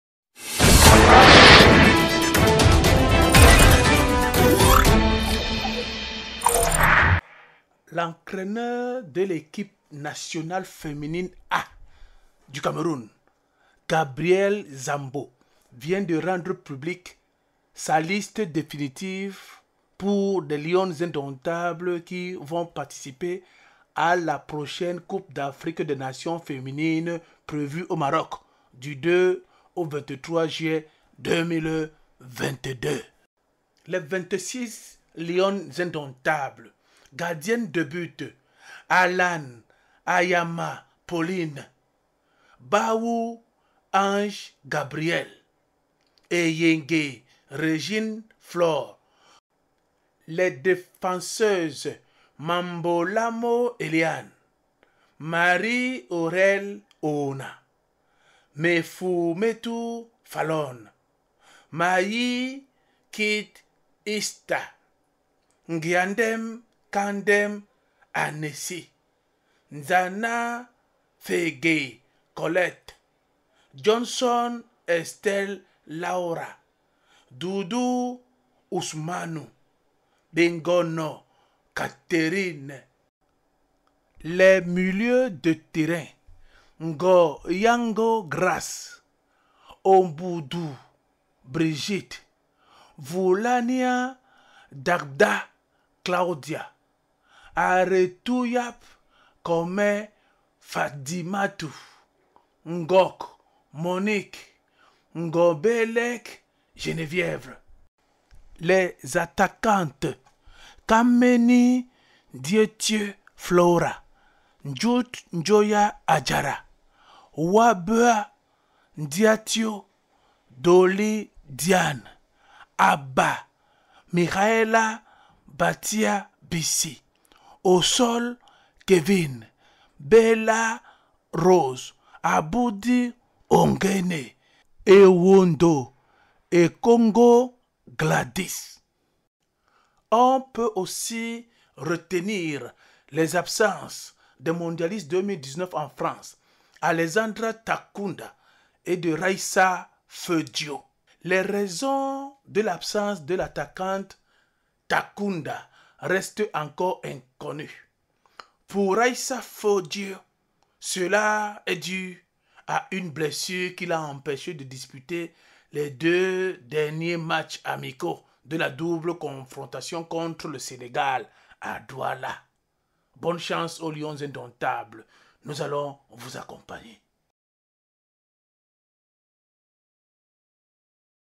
La liste definitive des Lionnes Indomptables qui seront à l'expédition de la Can 2022 au MAROC .
La liste definitive des Lionnes Indomptables qui seront à l'expédition de la Coupe d'Afrique des Nations féminine au Maroc ⚽️
●Le Cameroun évoluera dans la Poule B en compagnie de la Zambie, le Togo et la Tunisie. La compétition démarre le 2 juillet pour s’achever le 23 juillet prochain. Avant de mettre le cap sur le Maroc, les Lionnes Indomptables croiseront l’équipe de France à Beauvais le 25 juin prochain, ensuite Sarcelles.